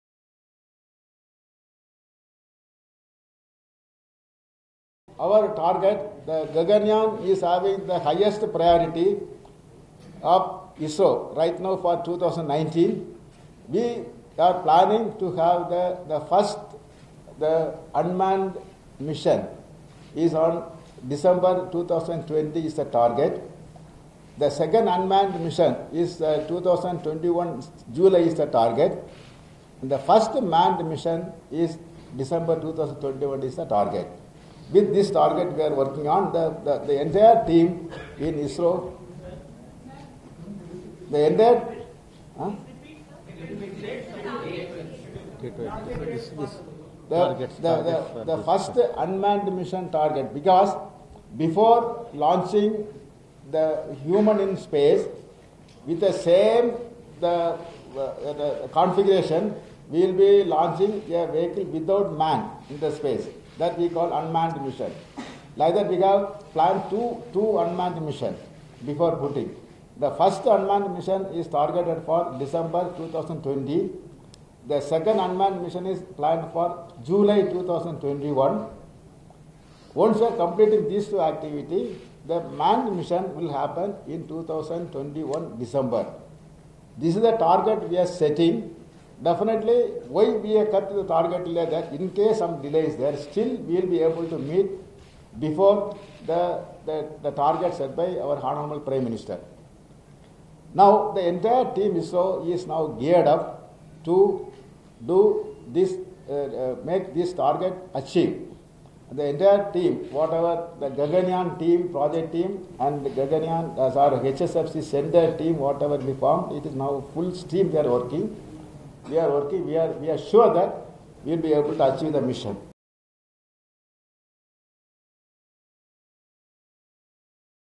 Gaganyaan Mission: First manned mission to space is set for December 2021, informs ISRO Chairperson
Bengaluru, Jan 11 (ANI): Indian Space Research Organisation (ISRO) is all set to launch India’s first human space flight – Gaganyaan Mission. K Sivan, chairperson of ISRO said, “The target for two unmanned missions to space is December 2020 and July 2021. The target for a manned mission to space is December 2021.” Once the first unmanned mission is completed, the manned mission (Gaganyaan) will be carried out.